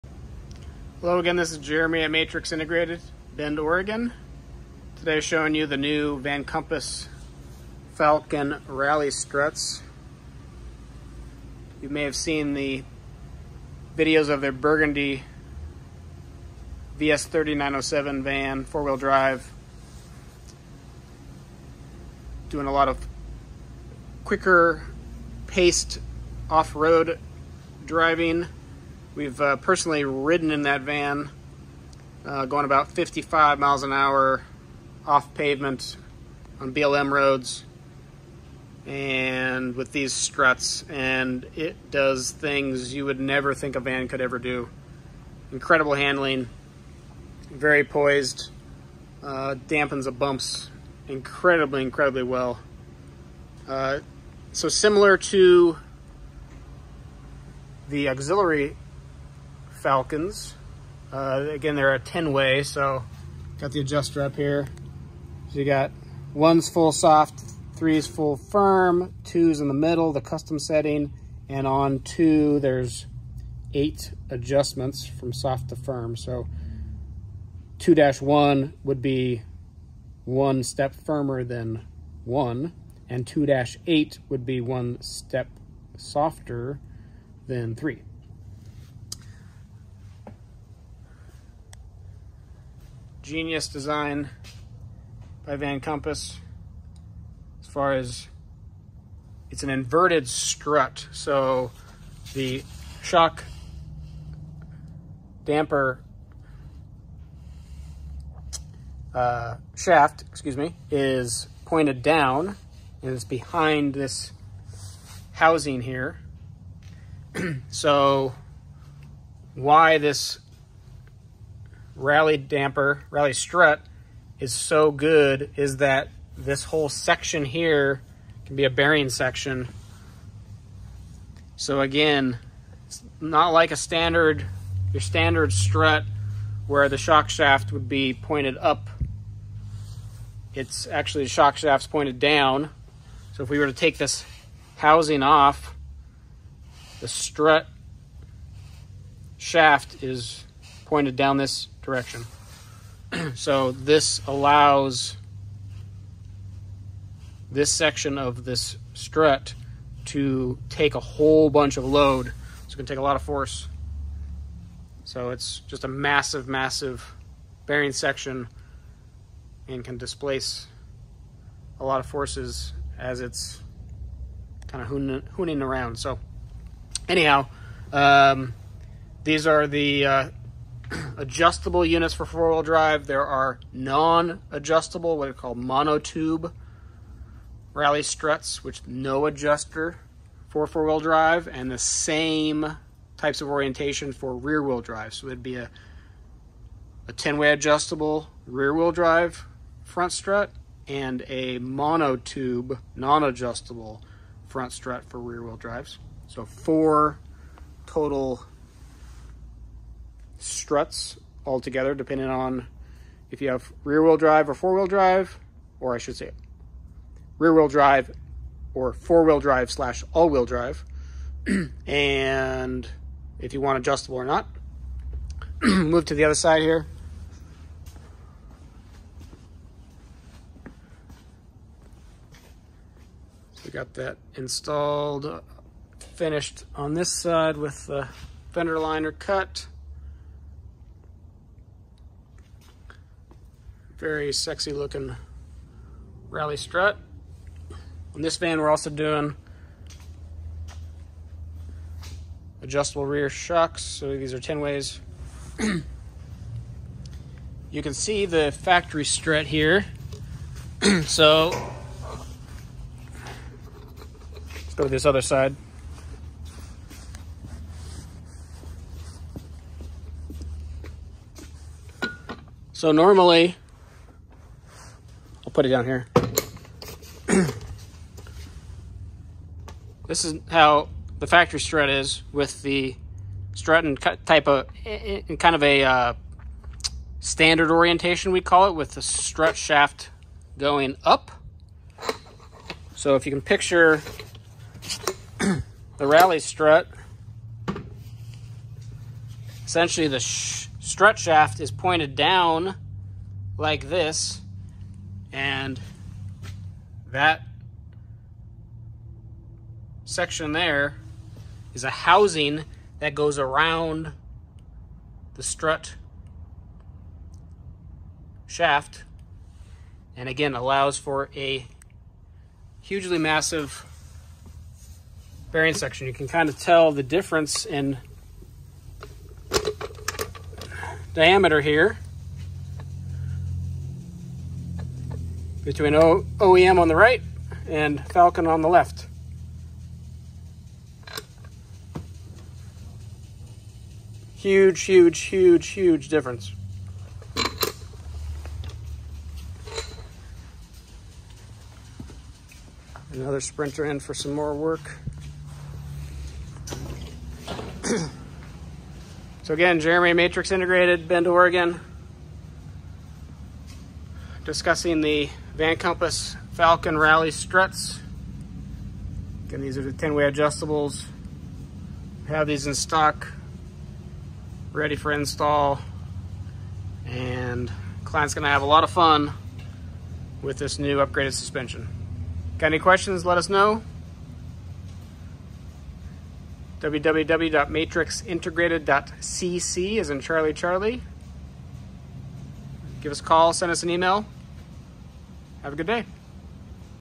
VanCompass inverted Rally Struts for Sprinter; mind-blowing performance
As the premier dealer/installer for Van Compass suspension products in the Pacific NW, check out the suspension upgrades Matrix Integrated’s Sprinter technicians are adding to this 2023 Sprinter 2500 AWD (VS30/907 chassis);

--Van Compass/Falcon 3.3 fast adjust inverted front Rally Strut, which is 10way compression adjustable
--VanCompass/Falcon 2.25" 3.3 fast adjust remote reservoir rear shock, which is 10way compression adjustable
--Sumo Spring front bump stops

VC’s all new inverted damper strut is produced from aerospace grade materials. Inverted dampers flip the idea of the conventional strut upside down, and use the damper body as the upper strut shaft, while the load bearing unit is contained in a massive lower housing that connects to the steering knuckle. This is the same application of technologies you will find on Dakar and World Rally Championship cars, which see some of the harshest driving conditions imaginable.

Major Benefits:
--Significantly increased load bearing surface area for long service life
--Damper shaft and seal surfaces are protected inside lower housing
--Much larger damping piston vs OEM
--Greater clamping force for alignment specs

Due to the strut's ideal position near the wheel, we are able to maximize the effectiveness of the 3.3 Fast Adjust cartridge. The result is a truly PHENOMENAL increase in damping performance, which translates to more stable and predictable handling, greater control across all conditions, and the ability to actually use the suspension to its full potential on and off road.

VC’s proprietary Rate Cup retains the OEM upper strut bushing for ease of installation and retention of OEM NVH characteristics. The patent pending Rate Cup preloads the bushing for tighter road feel, while dispersing the damping forces over the entire surface area of the bushing to prevent annoying clunks and premature bushing wear common with other solutions.

These struts are designed as a direct OEM replacement for any van, from stock passenger vans, luxury coaches and Class C RV's, to heavily modified adventure vans. They are compatible with stock height vans, and vans equipped with the VC Striker lift kits.

All of these components are available for both NCV3/906 (’07-18) and VS30/907 ('19+) chassis Sprinter. Professional installation of any of these upgrades (and many others) available at either our Bend or Portland, Oregon locations (no sales tax).